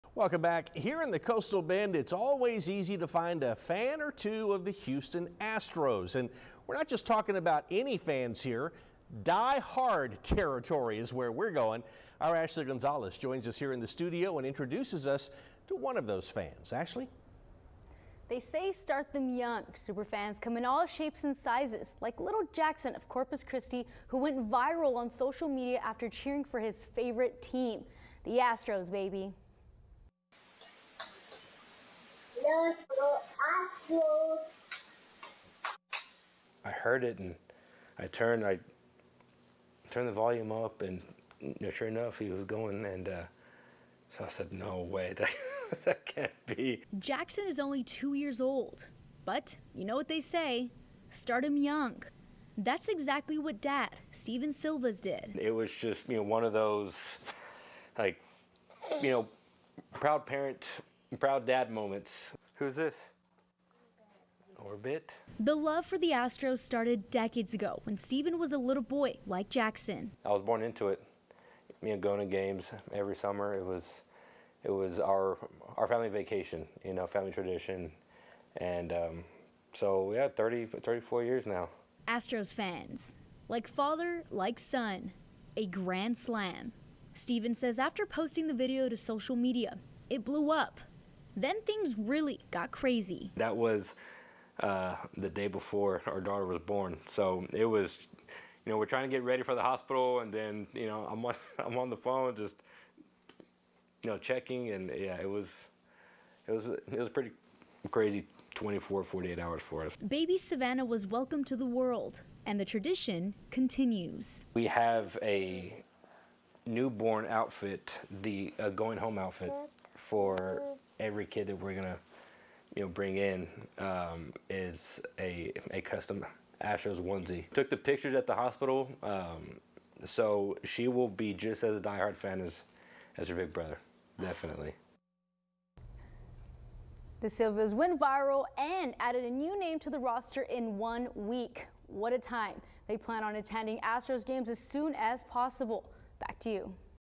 An Astros super from Corpus Christi goes viral
The Silvas went viral and added a new name to the roster in one week.